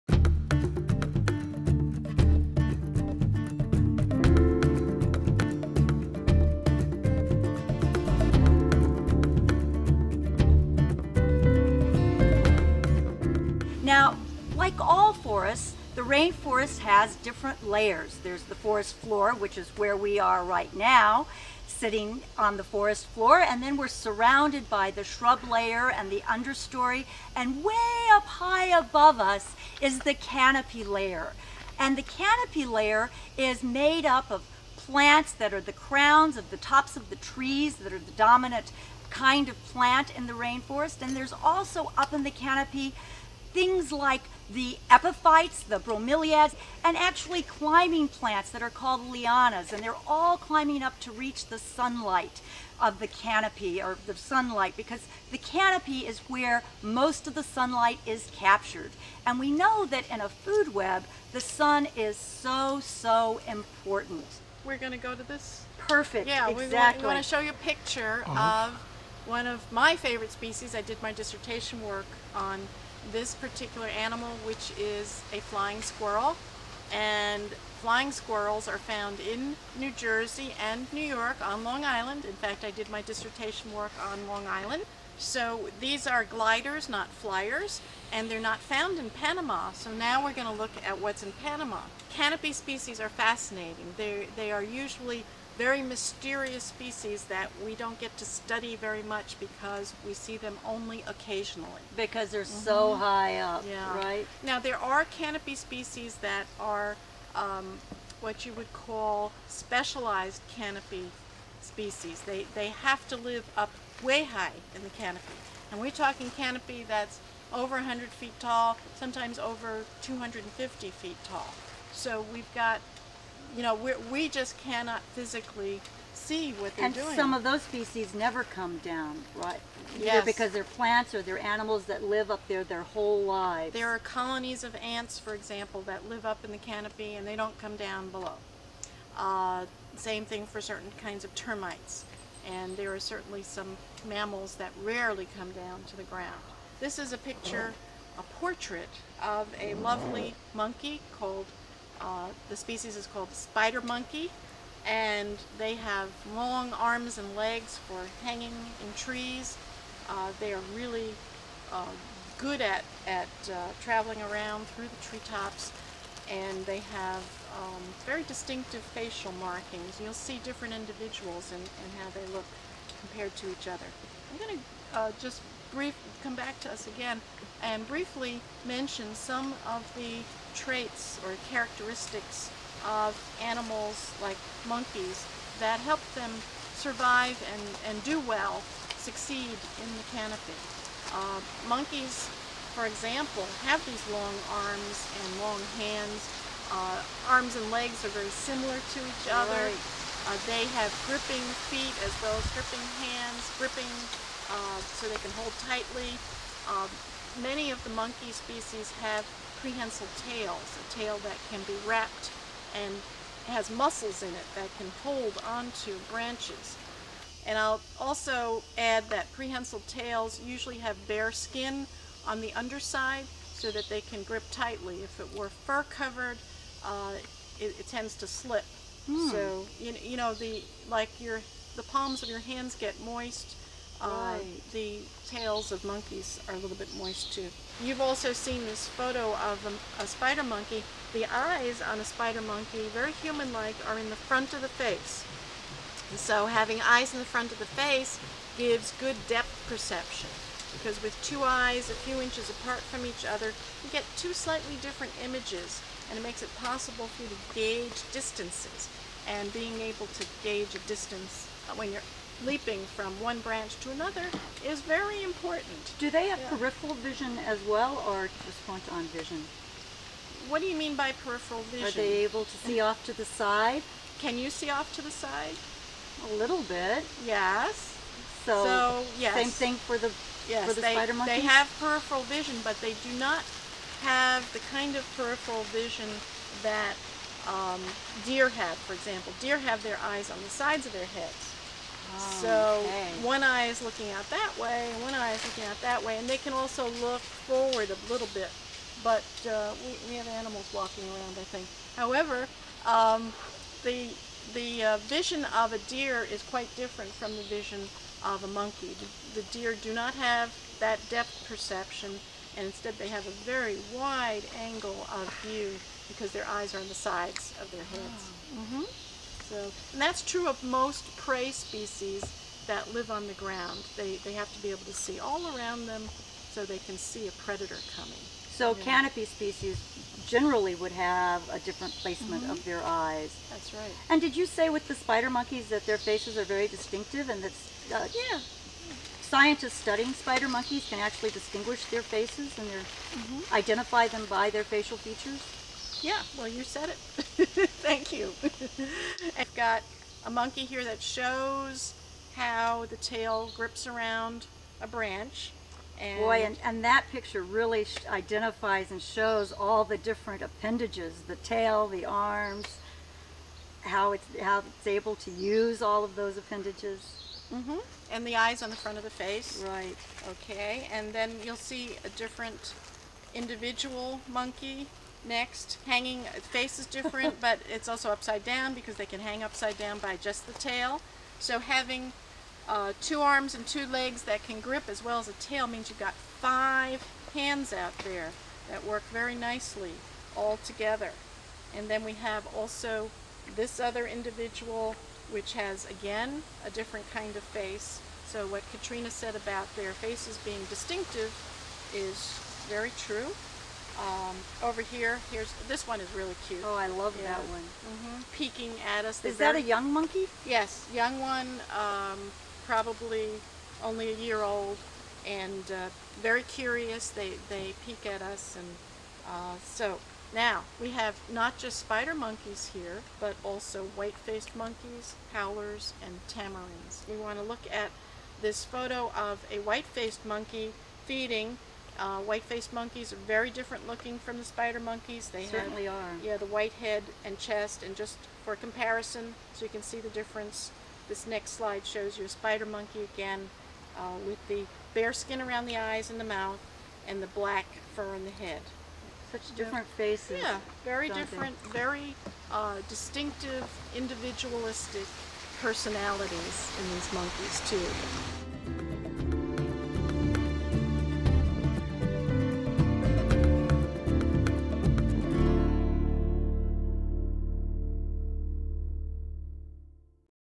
Canopy Species Part 1
Katrina Macht, M.A. and Jacalyn Willis, Ph.D discuss canopy species of Barro Colorado Island in Panama. They discuss the facial characteristics and adaptations needed to survive so far above the ground.